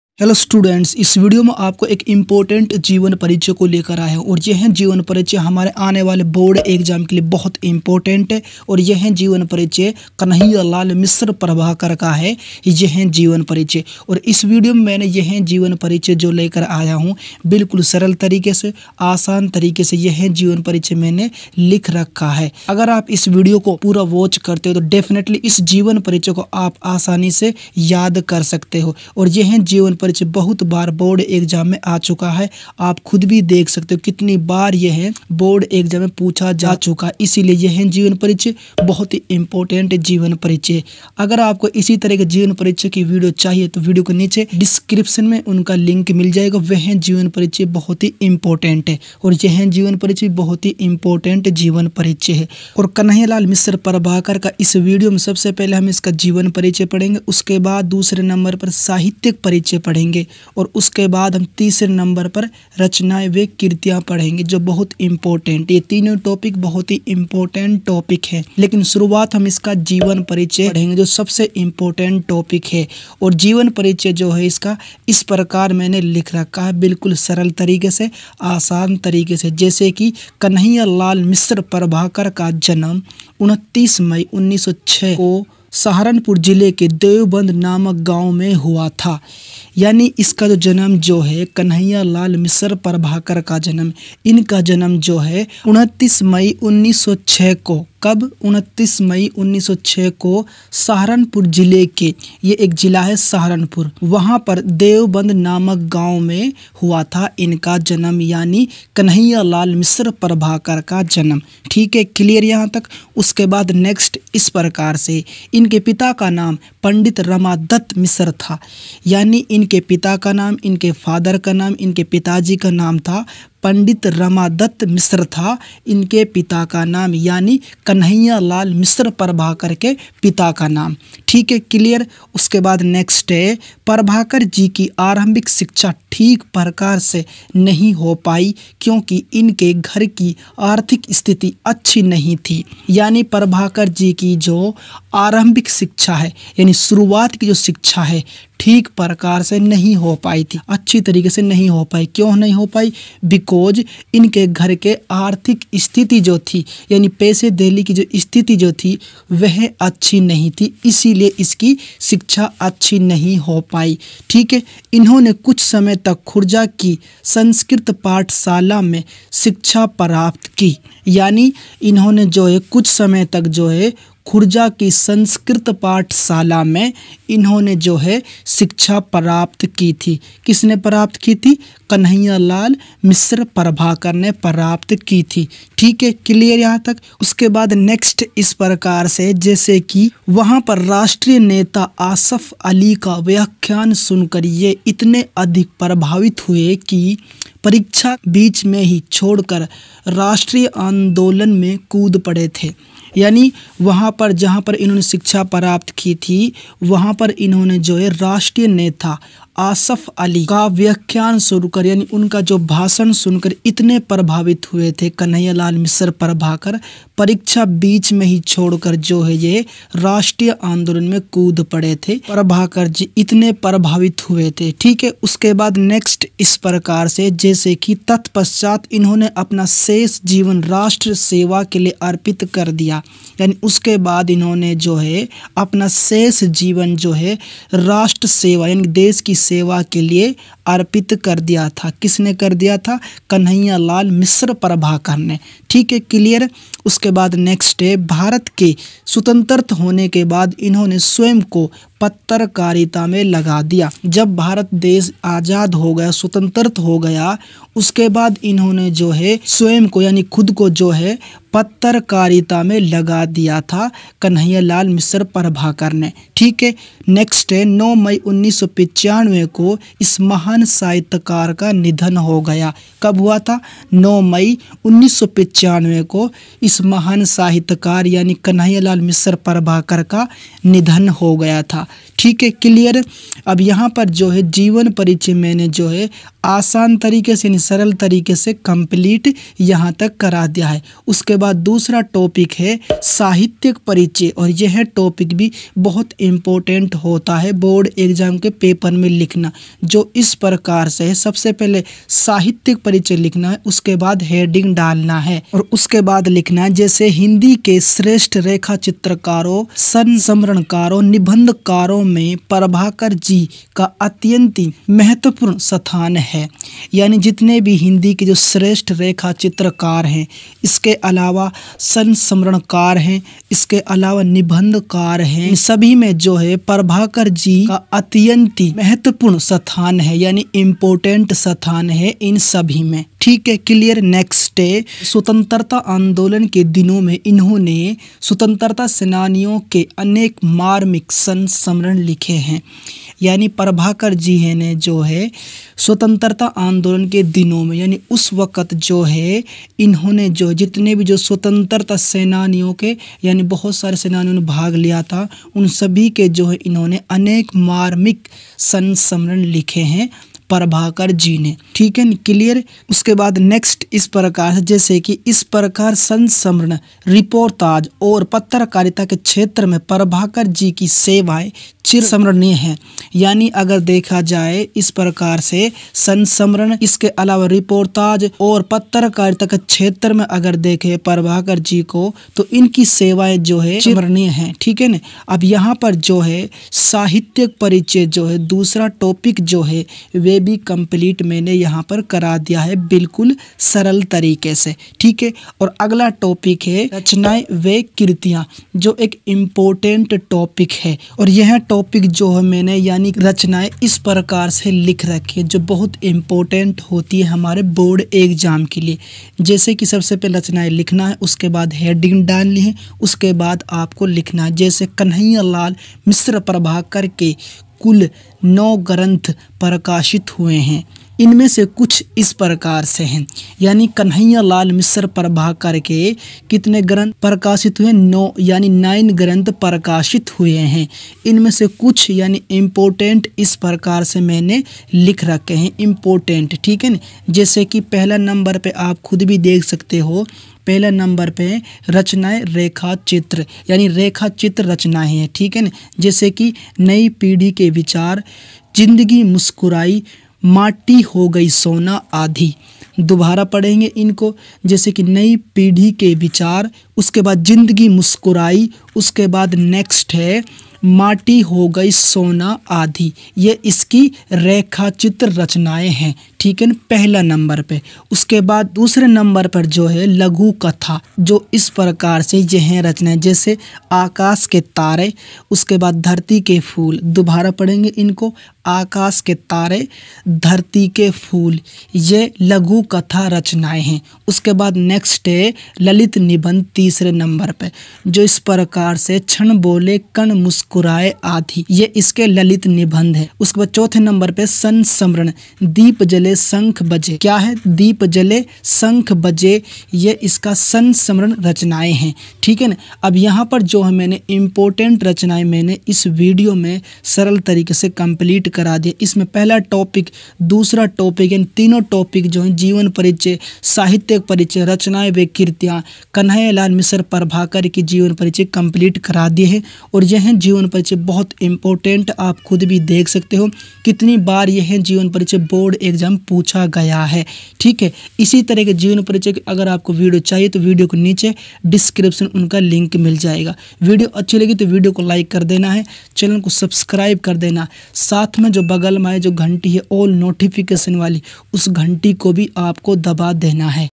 Kanhaiyalal mishra prabhakar ka jivan parichay | कन्हैयालाल मिश्र प्रभाकर का साहित्यिक/जीवन परिचय
Dear guys yeh video 12th class ke students very important video hai because isme 12th class ki book hindi usme ek important jivan parichay ko lekar aaya hun - aur yeh
kanhaiyalal mishra prabhakar ka jeevan parichay hai.

Topics Covered :-

1. कन्हैयालाल मिश्र प्रभाकर का जीवन परिचय
2.Kanhaiyalal mishra prabhakar ka jeevan parichay
3.jivan parichay kaise likhen